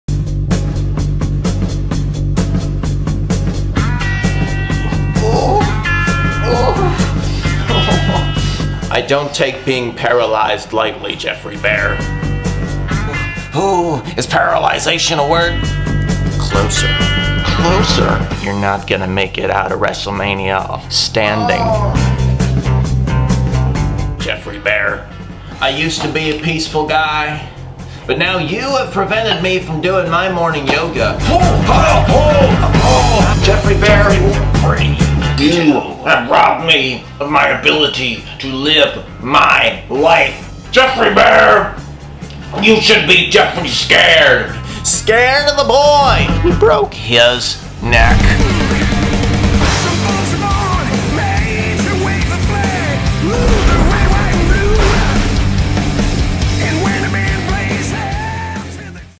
No Joke: Wrestlemania | Ultimate Challenge | Promo #2
Will Jeffrey Bare accept the Open Challenge? Will YOU accept the Open Challenge? February 15th @10pm in the Caplan Studio Theater! Special thanks to Kirill Bykanov!